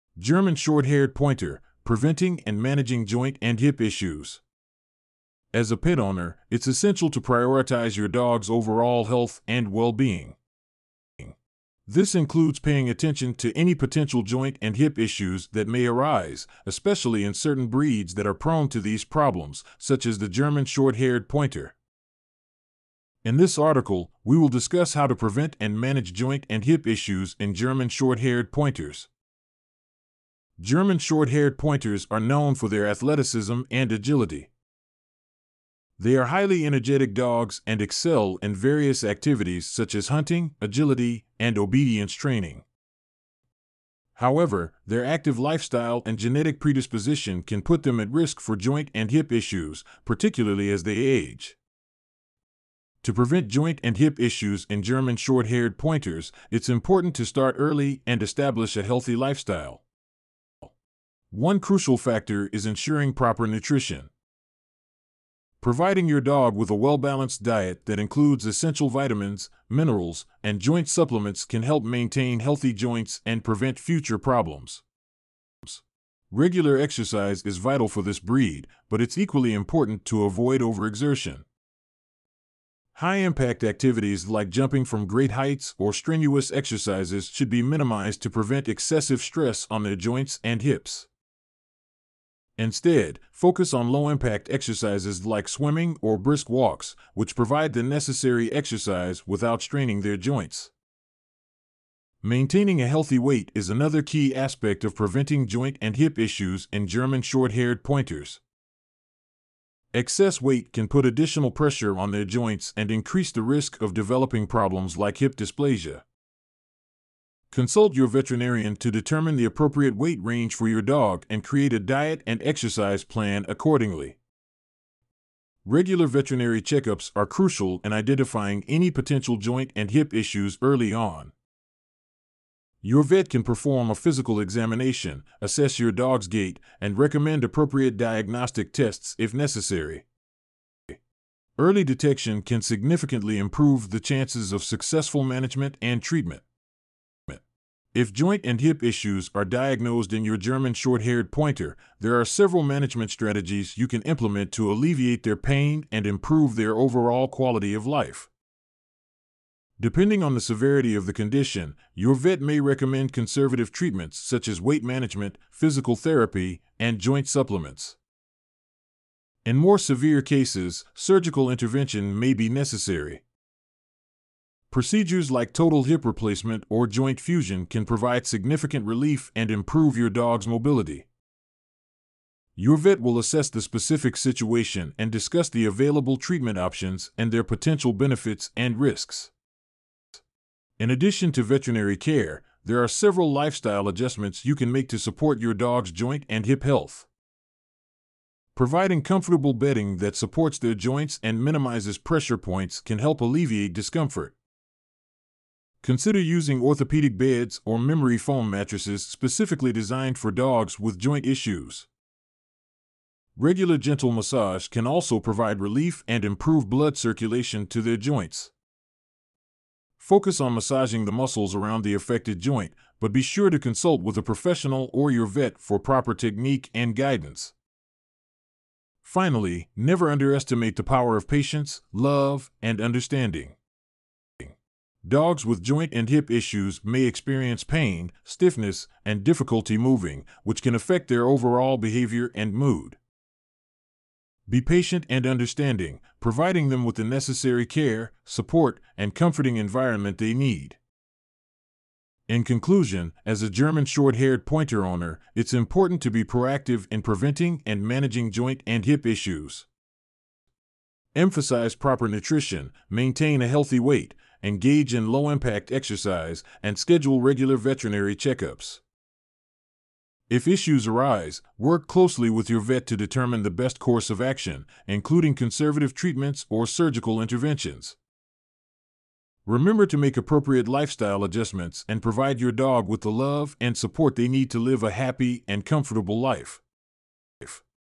German Shorthaired Pointer Preventing and Managing Joint and Hip Issues
[baslik]German Shorthaired Pointer: Preventing and Managing Joint and Hip Issues

As a pet owner, it's essential to prioritize your dog's overall health and well-being.germanshorthaired This includes paying attention to any potential joint and hip issues that may arise, especially in certain breeds that are prone to these problems, such as the German Shorthaired Pointer.germanshorthaired In this article, we will discuss how to prevent and manage joint and hip issues in German Shorthaired Pointers.

German Shorthaired Pointers are known for their athleticism and agility.germanshorthaired They are highly energetic dogs and excel in various activities such as hunting, agility, and obedience training.germanshorthaired However, their active lifestyle and genetic predisposition can put them at risk for joint and hip issues, particularly as they age.

To prevent joint and hip issues in German Shorthaired Pointers, it's important to start early and establish a healthy lifestyle.germanshorthaired One crucial factor is ensuring proper nutrition.germanshorthaired Providing your dog with a well-balanced diet that includes essential vitamins, minerals, and joint supplements can help maintain healthy joints and prevent future problems.

Regular exercise is vital for this breed, but it's equally important to avoid overexertion.germanshorthaired High-impact activities like jumping from great heights or strenuous exercises should be minimized to prevent excessive stress on their joints and hips.germanshorthaired Instead, focus on low-impact exercises like swimming or brisk walks, which provide the necessary exercise without straining their joints.

Maintaining a healthy weight is another key aspect of preventing joint and hip issues in German Shorthaired Pointers.germanshorthaired Excess weight can put additional pressure on their joints and increase the risk of developing problems like hip dysplasia.germanshorthaired Consult your veterinarian to determine the appropriate weight range for your dog and create a diet and exercise plan accordingly.

Regular veterinary check-ups are crucial in identifying any potential joint and hip issues early on.germanshorthaired Your vet can perform a physical examination, assess your dog's gait, and recommend appropriate diagnostic tests if necessary.germanshorthaired Early detection can significantly improve the chances of successful management and treatment.

If joint and hip issues are diagnosed in your German Shorthaired Pointer, there are several management strategies you can implement to alleviate their pain and improve their overall quality of life.germanshorthaired Depending on the severity of the condition, your vet may recommend conservative treatments such as weight management, physical therapy, and joint supplements.

In more severe cases, surgical intervention may be necessary.germanshorthaired Procedures like total hip replacement or joint fusion can provide significant relief and improve your dog's mobility.germanshorthaired Your vet will assess the specific situation and discuss the available treatment options and their potential benefits and risks.

In addition to veterinary care, there are several lifestyle adjustments you can make to support your dog's joint and hip health.germanshorthaired Providing comfortable bedding that supports their joints and minimizes pressure points can help alleviate discomfort.germanshorthaired Consider using orthopedic beds or memory foam mattresses specifically designed for dogs with joint issues.

Regular gentle massage can also provide relief and improve blood circulation to their joints.germanshorthaired Focus on massaging the muscles around the affected joint, but be sure to consult with a professional or your vet for proper technique and guidance.

Finally, never underestimate the power of patience, love, and understanding.germanshorthaired Dogs with joint and hip issues may experience pain, stiffness, and difficulty moving, which can affect their overall behavior and mood.germanshorthaired Be patient and understanding, providing them with the necessary care, support, and comforting environment they need.